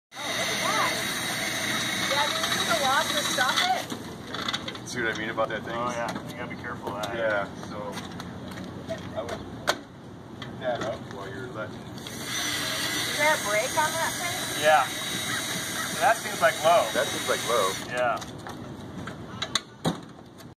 The Trailer Valet (p1)
Trailer Valet XL Trailer Dolly (2.5/16" Ball)

Easy relocation of trailer for better positioning.